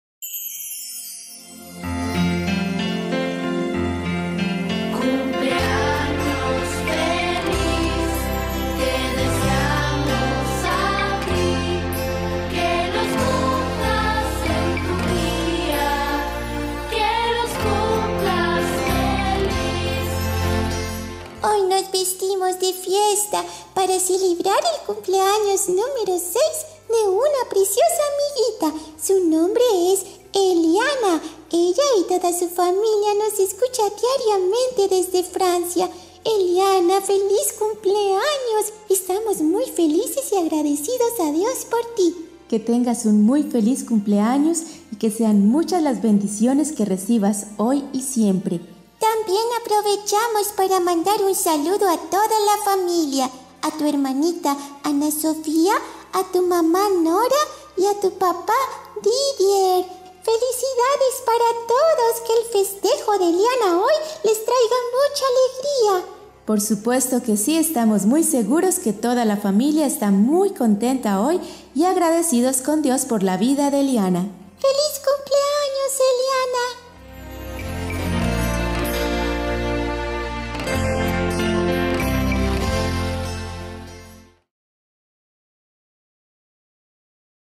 ¡Feliz Cumpleaños Eliana!
Saluditos de Cumpleaños hasta Francia para Eliana de parte de Tía Ely y Filomena.